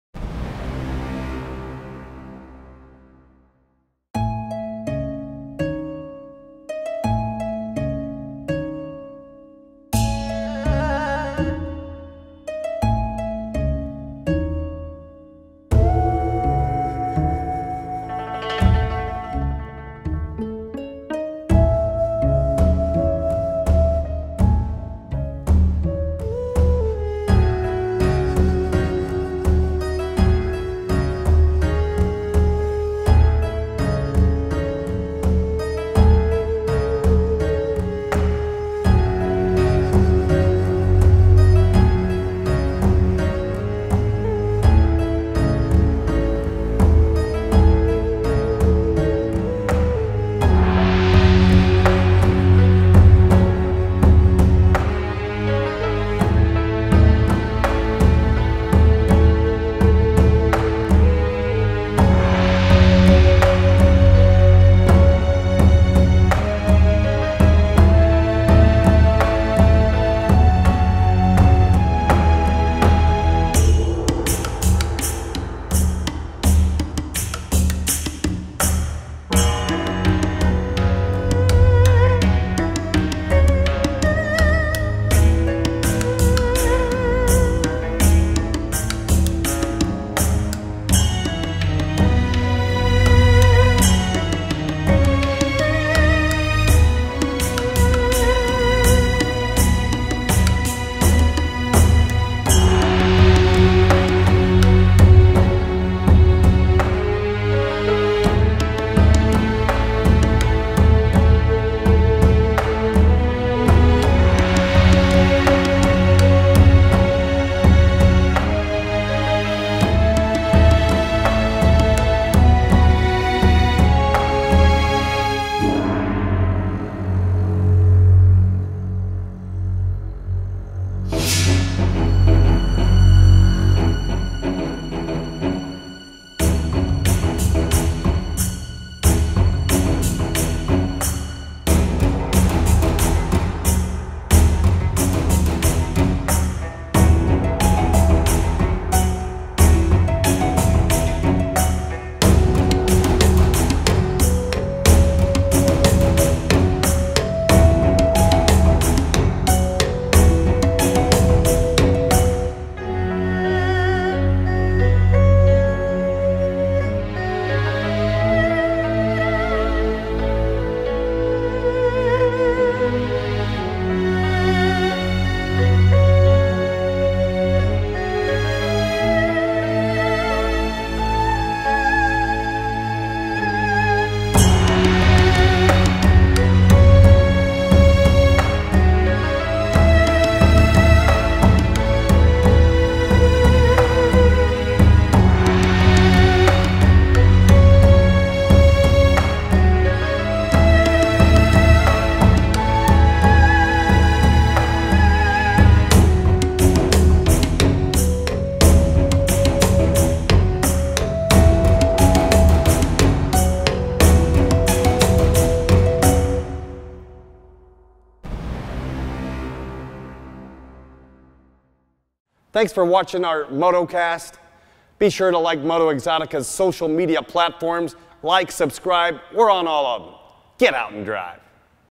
1991 Toyota Crown Hearse || SOLD
Seeking something unique to take to next year’s Halloween party?Need A unique vehicle to park in front of your restaurant. MotoeXotica Classic Cars presents this 1991 Toyota Crown Hearse. It was made in Toyota’s Motomachi, Japan factory. A converted commercial van, this example was assigned and hand built for funeral duty in Japan, so it’s a right-hand-drive vehicle and it is street legal to operate in the United States, with a clear, actual mileage VA title.

Finished in black (what else?) this hearse’s paint and trim are in very good order overall, with minor blemishes visible upon close inspection. The hand made craftsmanship on this unique vehicle is exceptional. It is all hand made wood construction with hand laid copper roof and tooled metal detail.

The bodywork is in great overall condition, the engine bay is quite tidy, the battery is in very good order, the cargo area is functional for caskets to make your next Halloween that much more macabre. The chrome bumpers are in satisfactory order.

In that engine bay is a 2.0L overhead cam, straight six-cylinder engine buttoned to a five-speed manual transmission.

This Crown rolls on Yokohama radials, size 185R14 at all four corners. Each donut surrounds a steel wheel topped by factory wheel covers. The tires are in satisfactory order while the wheel covers are in very good condition.

Inside, the cab is finished in navy blue. The vinyl bucket seats are in great shape, as are the black floor mats. The headliner and two-spoke steering wheel are also in very good shape. The instrument panel and inner door liners are in better-than-average shape. The center console is in satisfactory condition, as is the shift lever. A factory AM radio completes the cabin.

Whether you’re looking for a rare rolling billboard for your business, or just to indulge in some fun for a future Halloween, this early 90s Toyota is a special way to fulfill those desires. Take it to a car show in most of North America and you’re virtually guaranteed not to encounter another while at the show. Stop by MotoeXotica Classic Cars and check this one out for yourself, if you dare.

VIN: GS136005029

This car is currently located at our facility in St. Louis, Missouri. Current mileage on the odometer shows 36,934 miles or 59,441 kilometers. It is sold as is, where is, on a clean and clear, actual mileage VA title. GET OUT AND DRIVE!!!